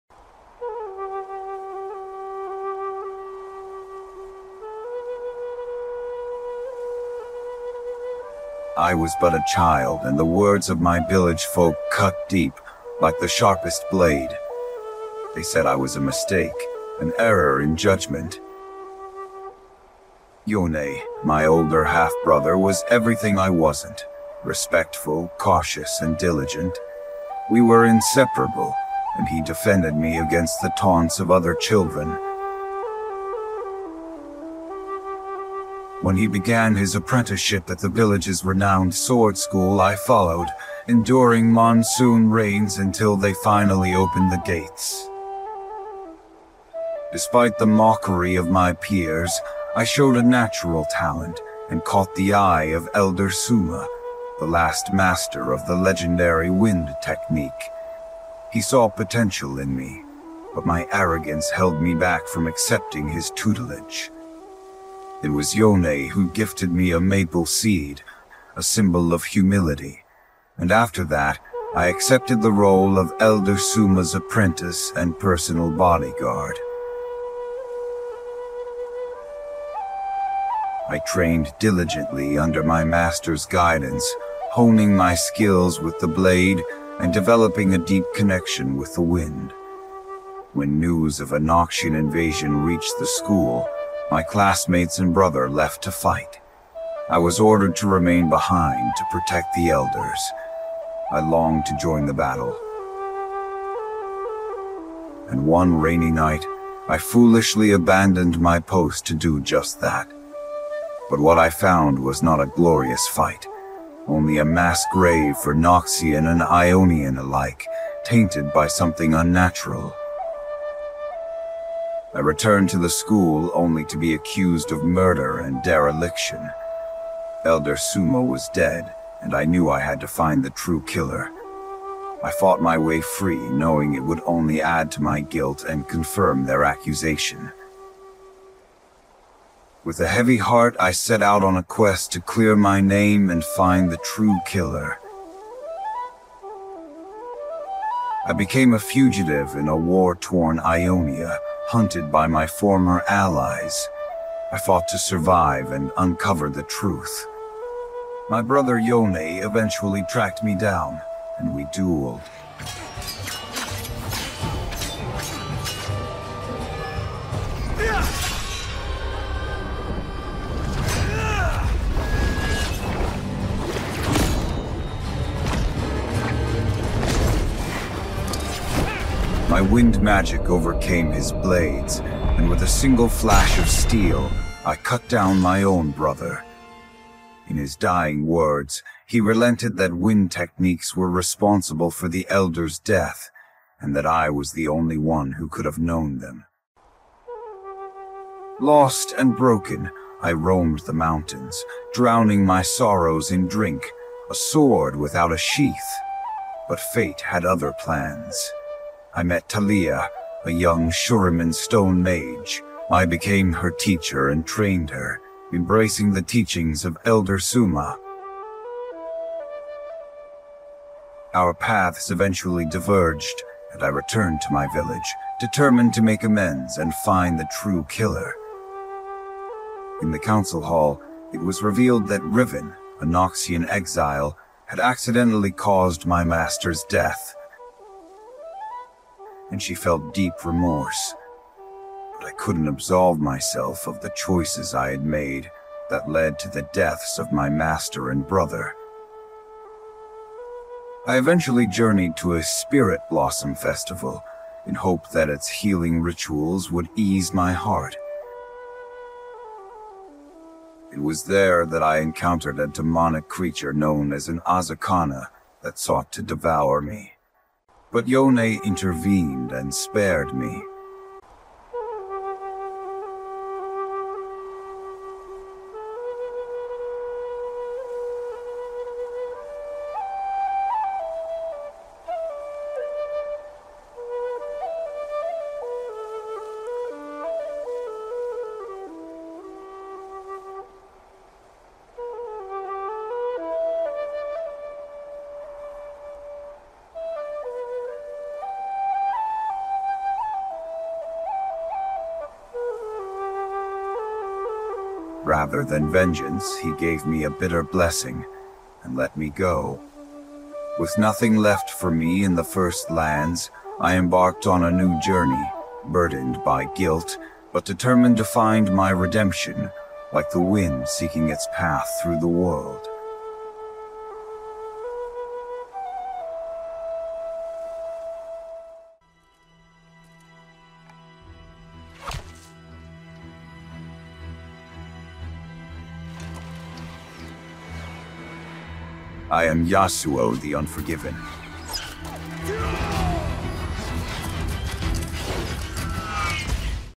Yasuo Narrates His Story In First Person
From his childhood in a small Ionian village to his quest for justice and redemption, discover the twists and turns of his life, the bonds he formed, and the challenges he faced, all while striving to clear his name and find the true killer of his master.